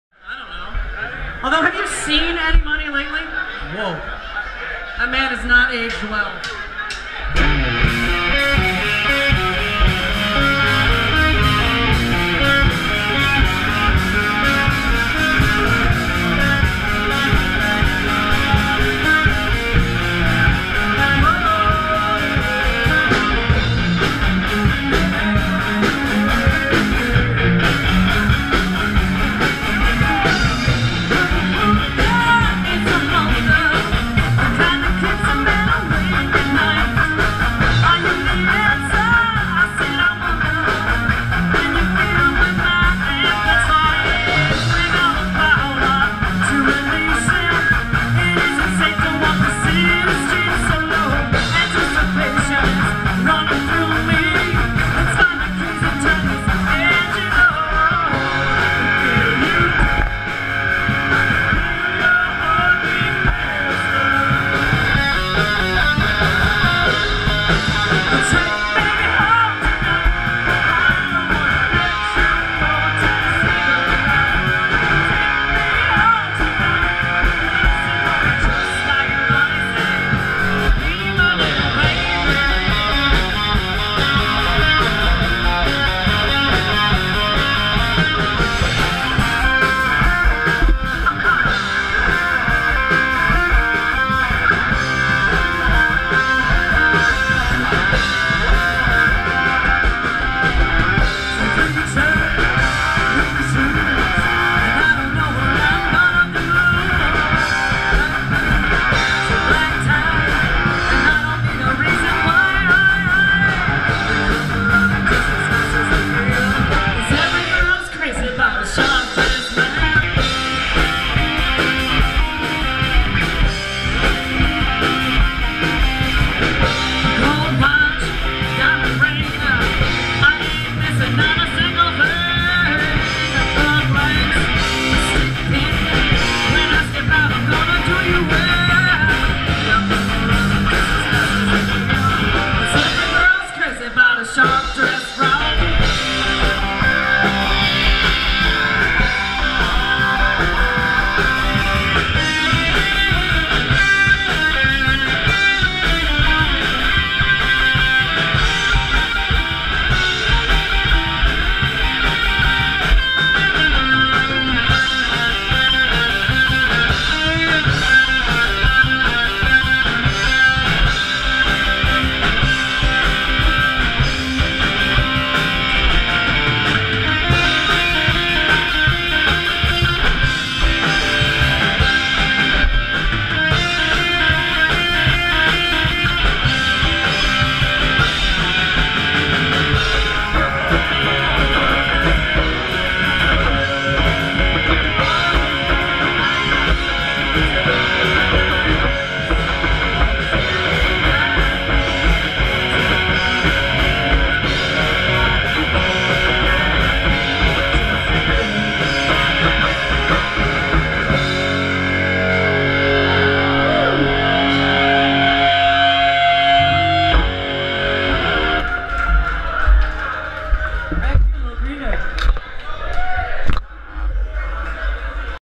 Pathetic Cam - Avenue Tap House (Mike Layton)
Pathetic Cam Video 1. Live at Avenue Tap House in Lakewood, Ohio. With Mike Layton.